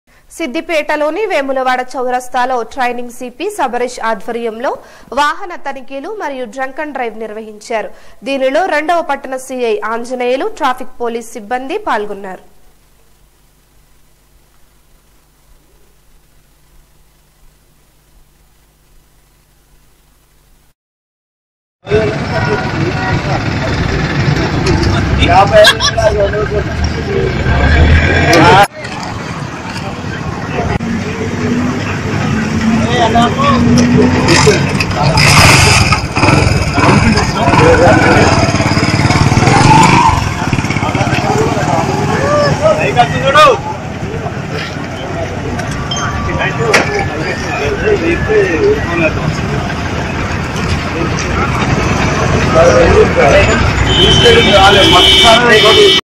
సిద్దిపేట లోని వేములవాడ చౌరస్తా లో ట్రైనీ cp శబరిష్ ఆద్వర్యమ్ లో వాహనాల తనిఖీ
సిద్దిపేట లోని వేములవాడ చౌరస్తా లో ట్రైనీ cp శబరిష్ ఆద్వర్యమ్ లో వాహనాల తనిఖీ మరియు డ్రంక్ అండ్ డ్రైవ్  నిర్వహించడం జరిగింది.. దీనిలొ 2వ పట్టణ ci ఆంజనేయులు,  ట్రాఫిక్ పోలీస్ సిబ్బంది పాల్గొనడం జరిగింది